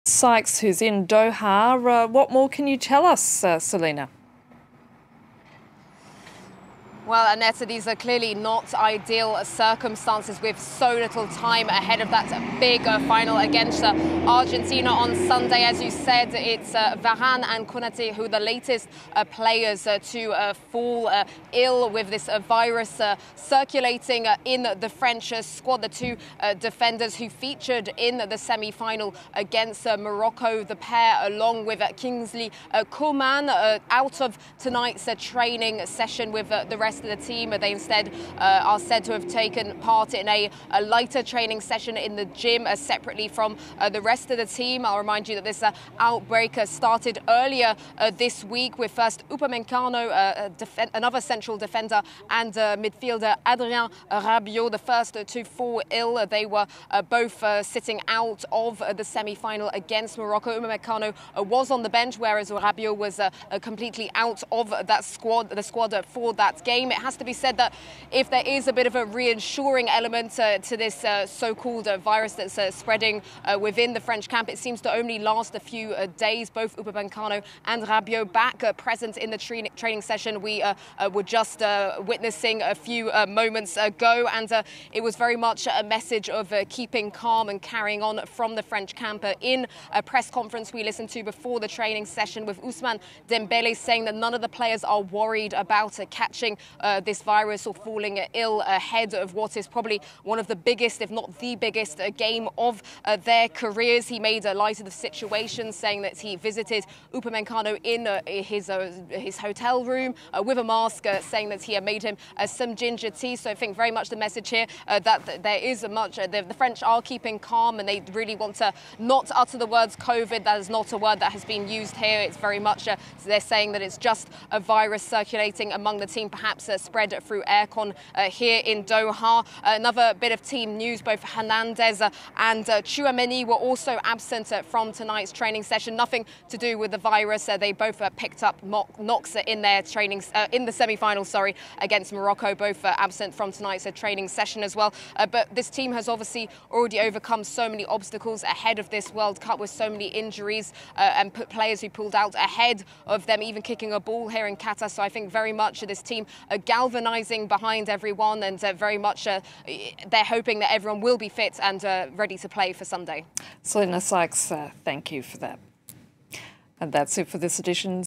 ‘Bad news ahead of the final’: Mystery illness afflicts France players at World Cup • FRANCE 24
Several France players have caught colds, the French Football Federation said, as the team prepares for the World Cup final against Argentina, with defenders Raphaël Varane and Ibrahima Konaté the latest to fall ill on Friday. FRANCE 24’s special correspondents report about this mystery illness from Qatar.